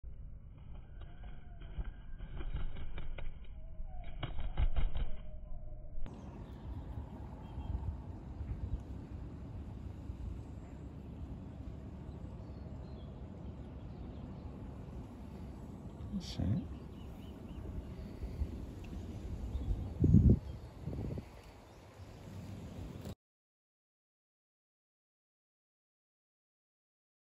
A robin eating from my hand
There's more to fishing that catching fish, and my bait on this winter day was tempting enough for this robin to come down and land on my hand once I sat still enough and quiet enough, for long enough.

Apparently robins in mainland Europe are far more shy than they are in the UK.  Perhaps this is because of the long standing relationship between angler and robin?